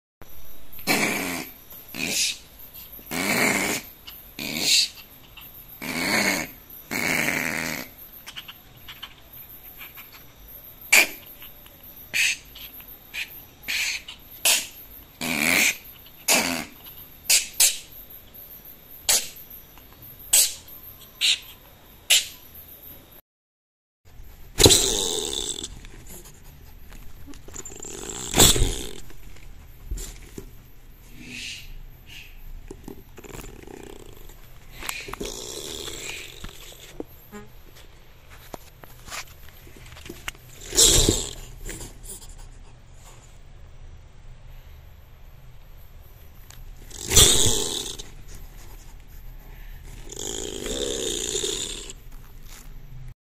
GRUÑIDO DE CHUCHA/ZARIGÜEYA/TLACUACHE 🐭 | ABRIL MARA
¿Habías escuchado antes este sonido? es el gruñido de una zarigüeya. No te preocupes si las escuchas gruñir, aunque pueden sonar un tanto agresivas, son animales muy nerviosos que únicamente quieren huir. Dales su espacio y permíteles seguir su camino.